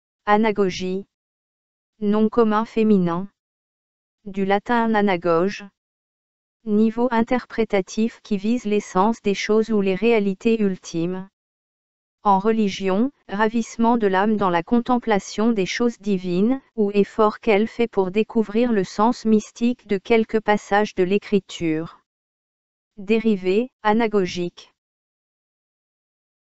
Anagogie | Dictionnaire français audio
Définition du terme Anagogie.
Nom commun féminin.
Du latin anagoge.
Niveau interprétatif qui vise l’essence des choses ou les réalités ultimes.
En Religion, Ravissement de l'âme dans la contemplation des choses divines, ou efforts qu'elle fait pour découvrir le sens mystique de quelques passages de l'Écriture.
Dérivé : anagogique.